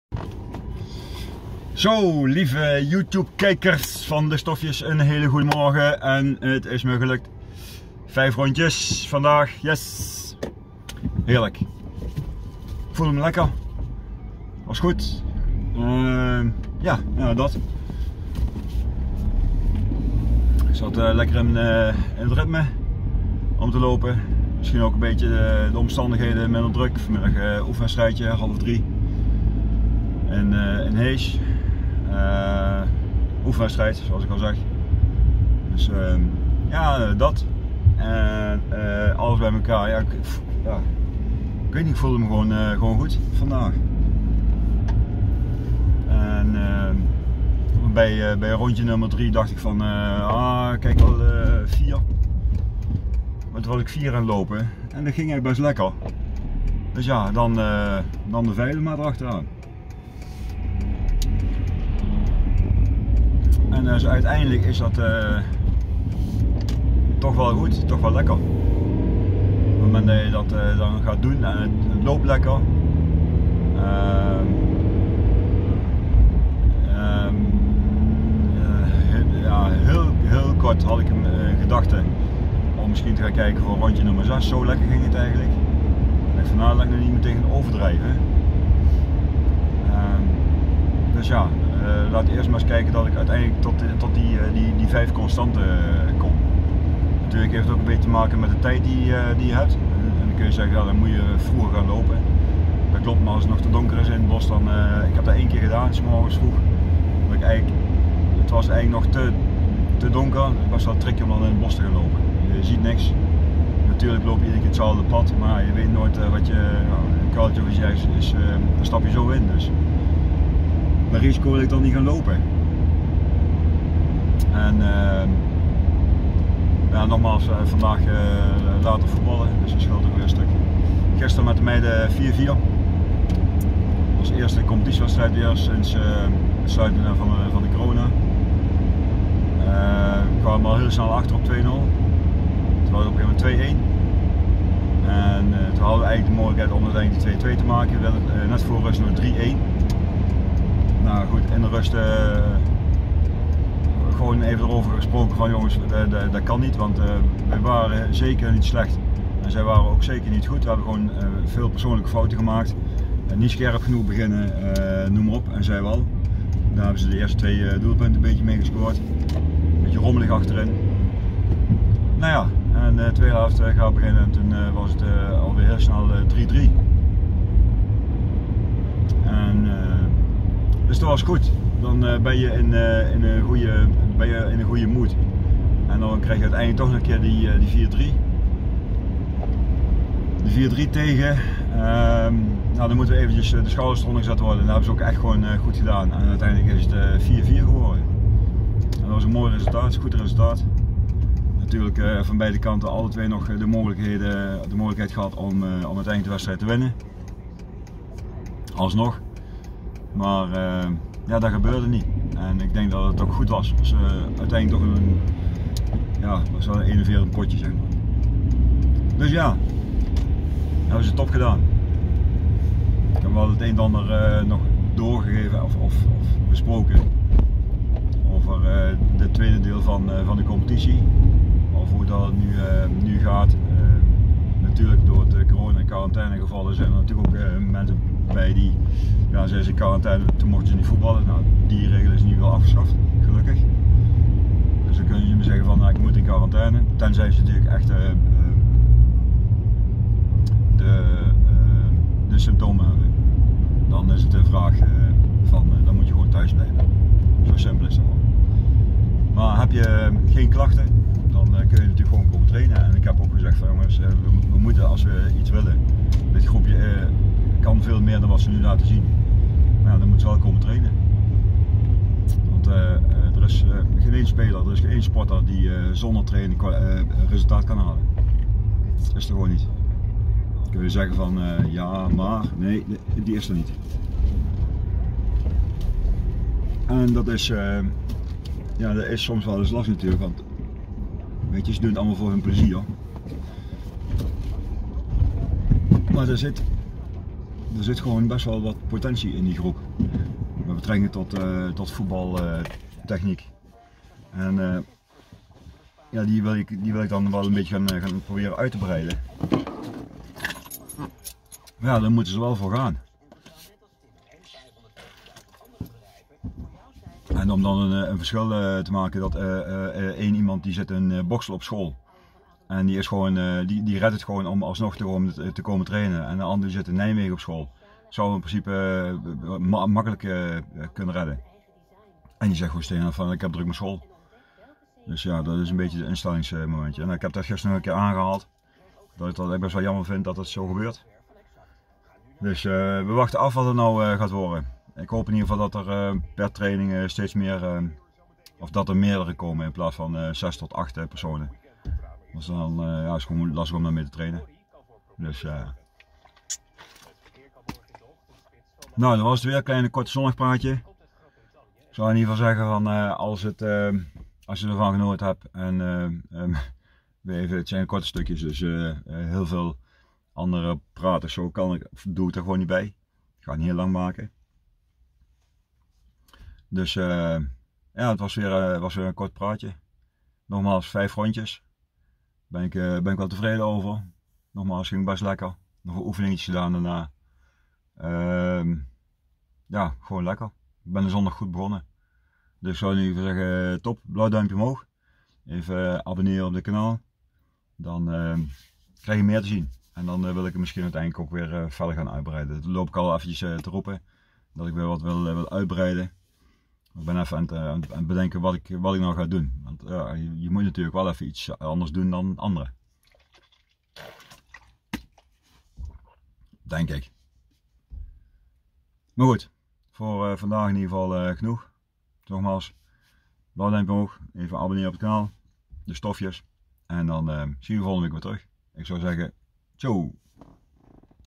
Vandaag lekker 5 rondjes gedaan #41
He, dat was fijn het ging echt lekker vandaag. Dan maakt het ook niet uit wat de tijd is. Maar zelfs die was nog best goed. Al met al fijn gelopen en een lekker begin van de dag.